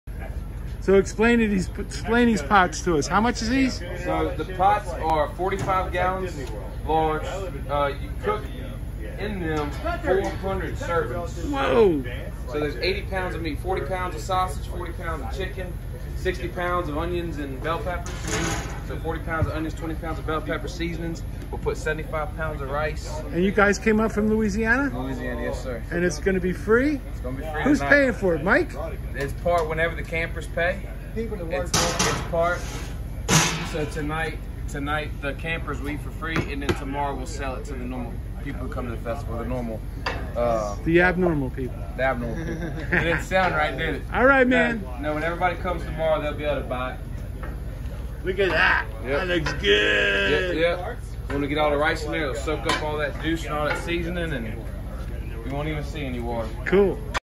Cooking up the free jambalaya
All the Crawfish fest campers will be treated to free jambalaya made by a great group of folks who came up from Louisiana. Looking forward to trying it out at 5:30z